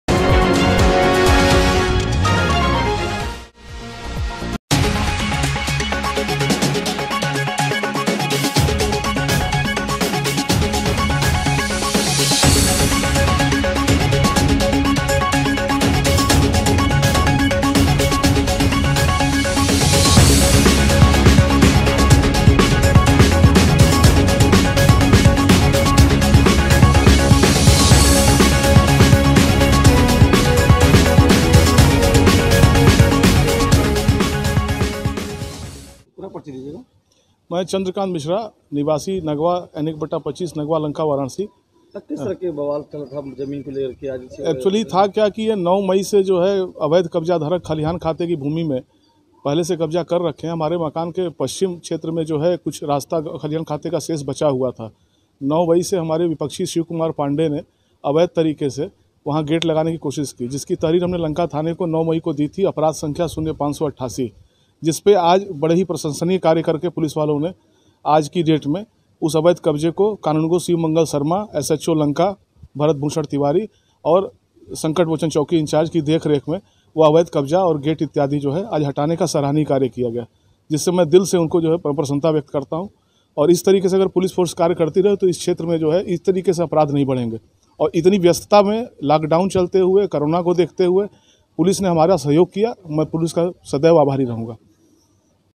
पुलिस ने खाली कराई एक साल से कब्जा की गई जमीन
वाराणसी - नगवा लंका थाना एस एच ओ भारत भूषण तिवारी एवम् राजस्व निरीक्षक शिवमंगल शर्मा की देख रेख़ एवम् दिशा निर्देशन में दिनांक 9 मई 2019 से शिवकुमार पांडेय पुत्र स्वर्गीय शंभूनाथ पांडेय द्वारा नगवा रविदास पार्क के मुख्य मार्ग पर किए गए खलिहान खाते की जमीन पर अवैध कब्जे जिसमे दिनांक 18/19 मई की रात लगाए गए गेट निर्माण को प्रार्थी चंद्रकांत मिश्रा के शिकायत पर उक्त कब्जे को हटवाया गया । जो की थाना लंका एवम् राजस्व निरीक्षक ने Covid 19 के ड्यूटी के दौरान सराहनीय कार्य किया।